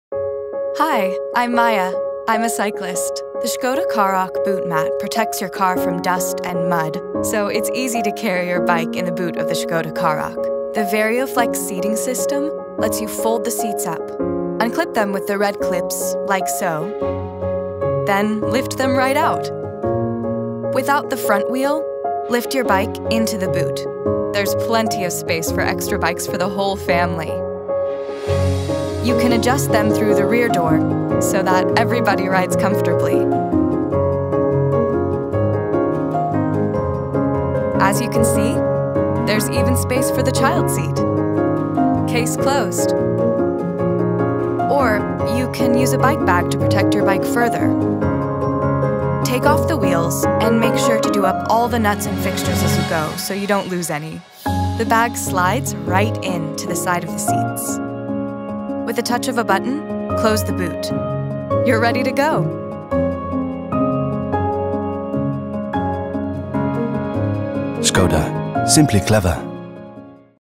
Dlatego Jeździmy!: Jak korzystać z systemu VarioFlex?
System VarioFlex w Twojej ŠKODZIE KAROQ pozwoli Ci z łatwością przewieźć rower w bagażniku. Dowiedz się więcej o tym rozwiązaniu